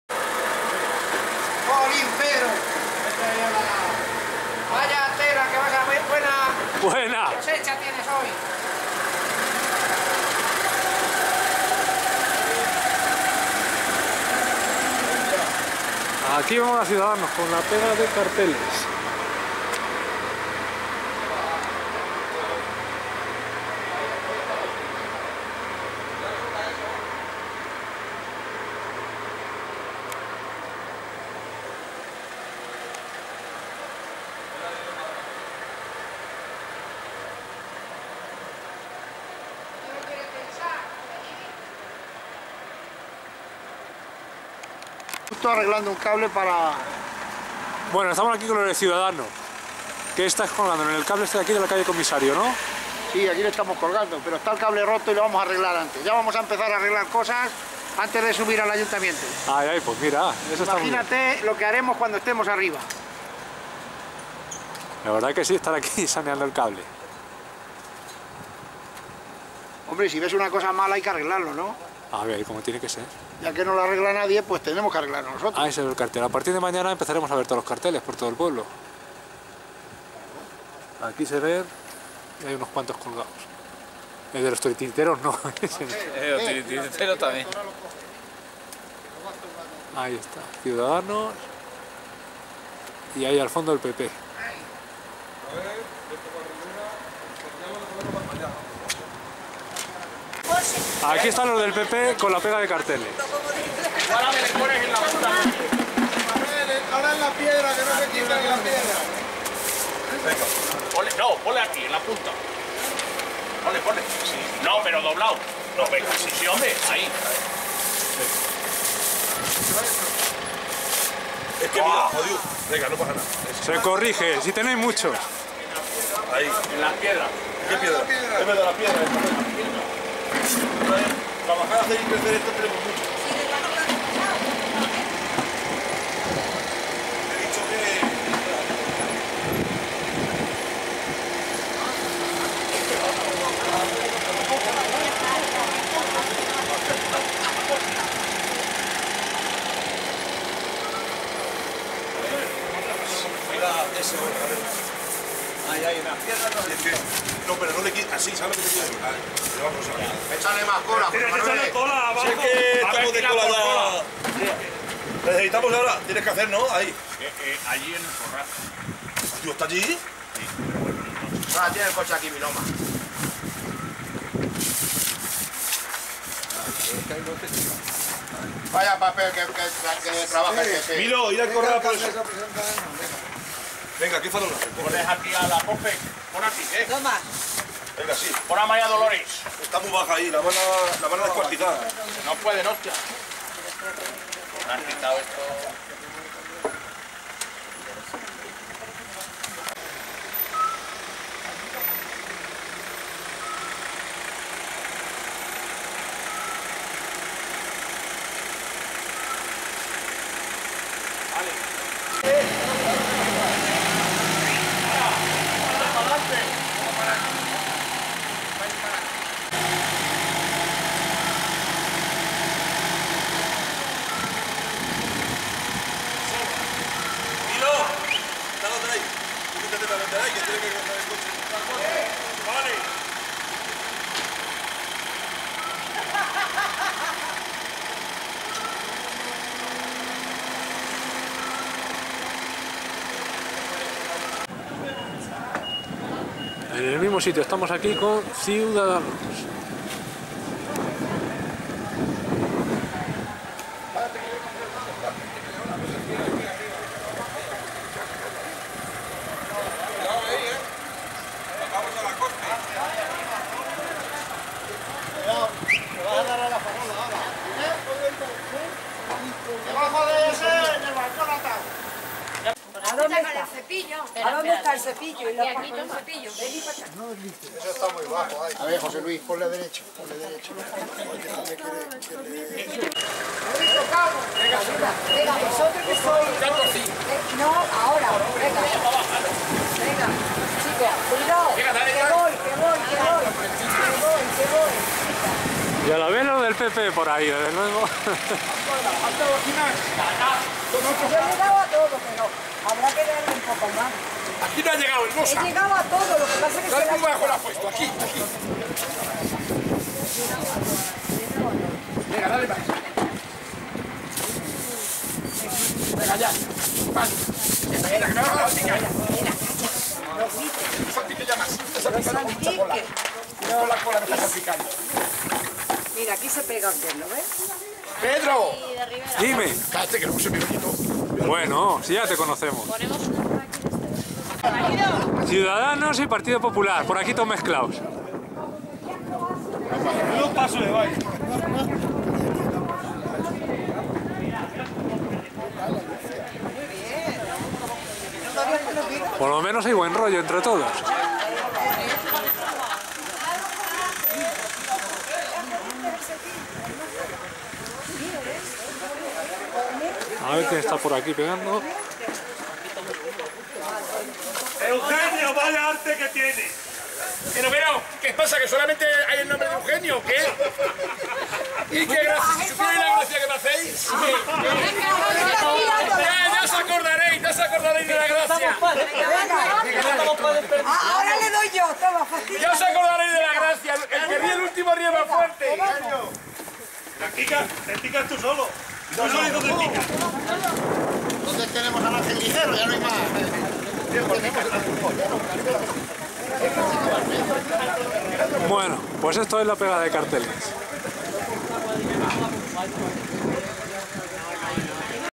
Pegada de carteles en la apertura de campaña electoral de Tembleque. Elecciones Municipales 2015
Los partidos CIUDADANOS y PARTIDO POPULAR, ambos de Tembleque, pasada la medianoche del día 8 de mayo comenzaron con la tradicional pegada de carteles electorales por las calles del pueblo. También algunas pancartas colgadas en distintos puntos.